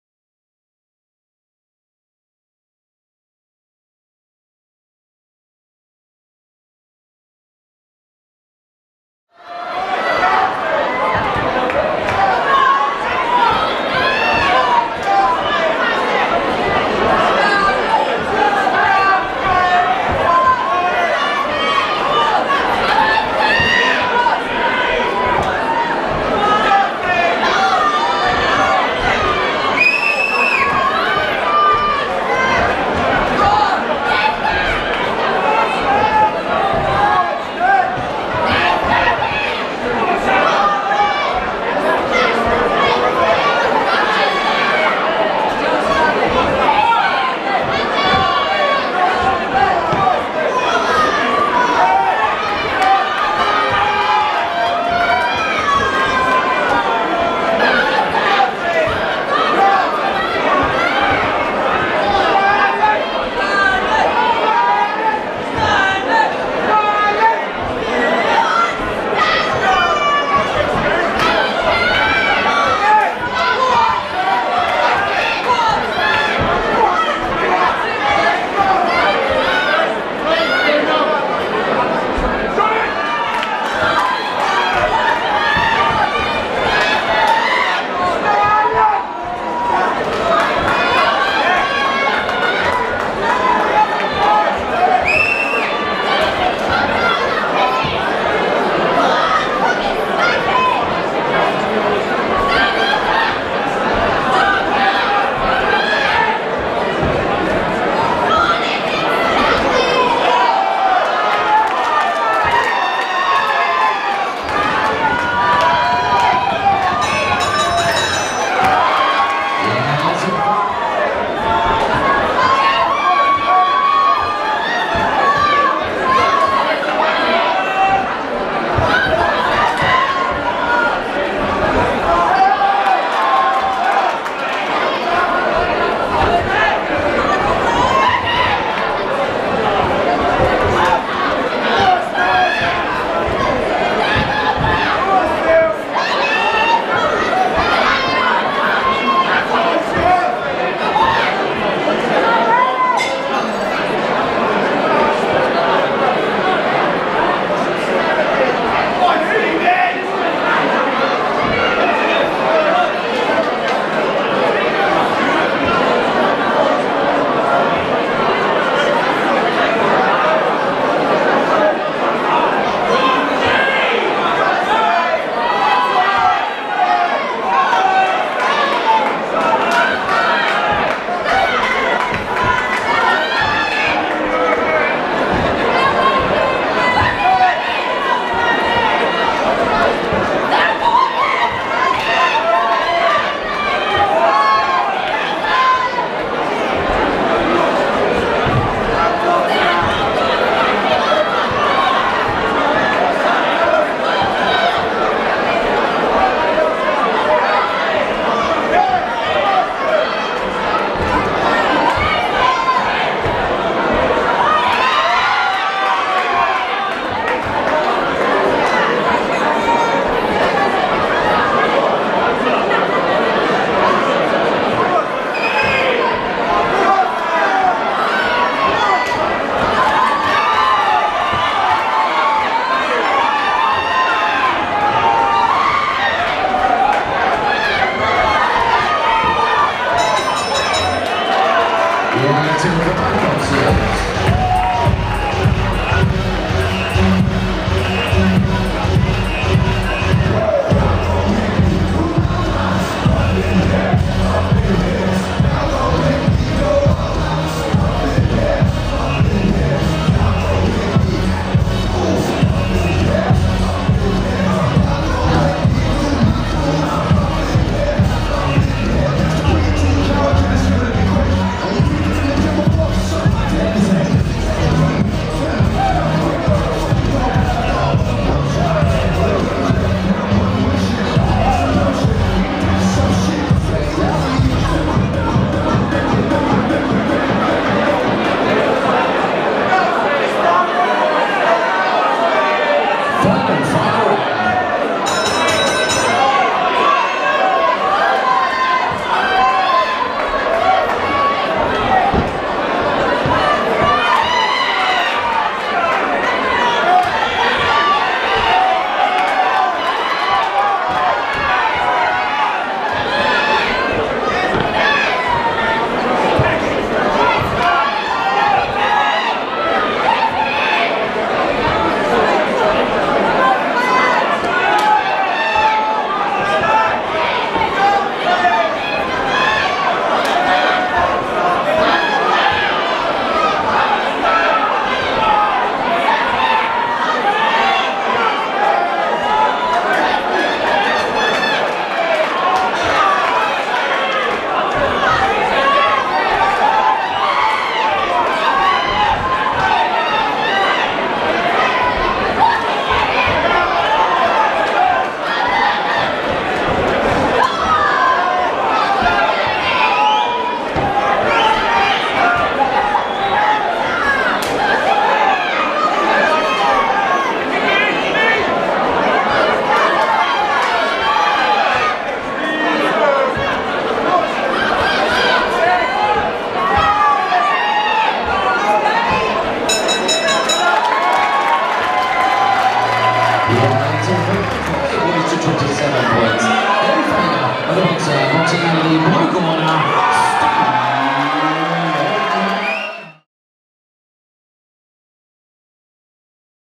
Ultra White Collar Boxing | Brighton | Stanley Steere VS Steve Harlow
Brighton Ultra White Collar Boxing 3rd December 2017

8 Weeks FREE training / Get in the best shape of your life/ Raise money for Cancer Research UK